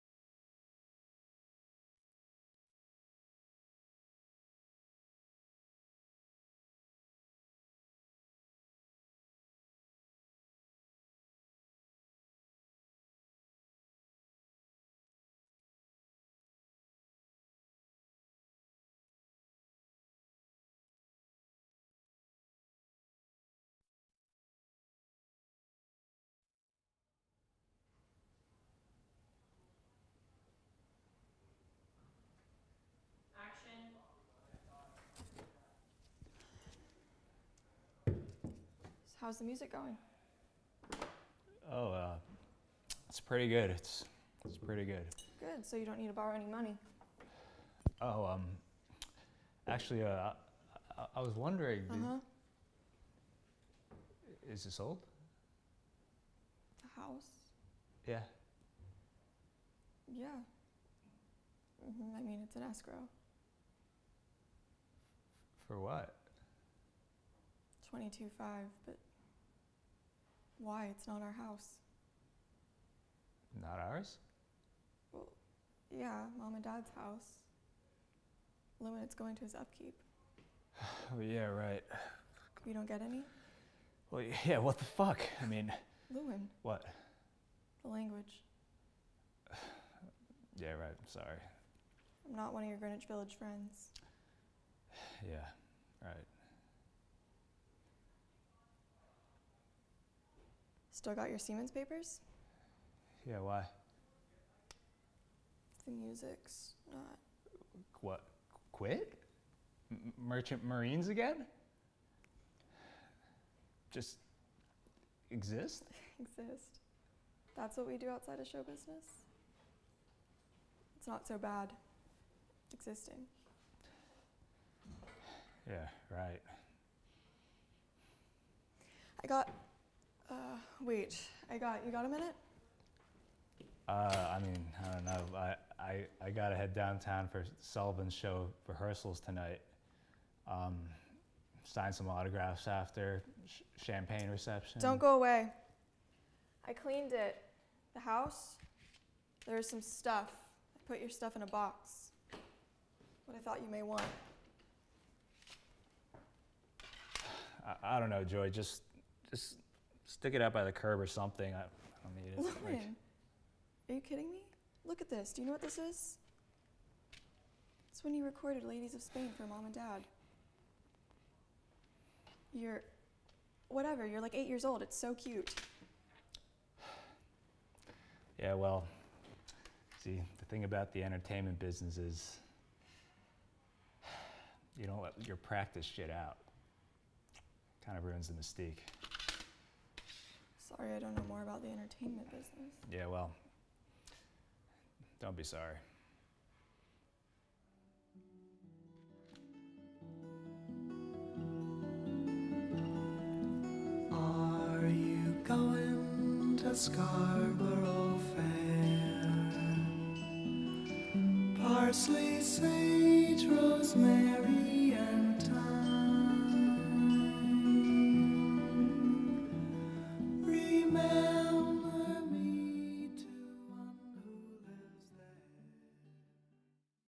Inside Llewyn Davis @ NYU Tisch (check my other scenes not this! smh)
Hi strangers the algorithm has brought you to one of my worst and first scenes lol. i had 1 hour notice to prepare for this via director too.  please my other work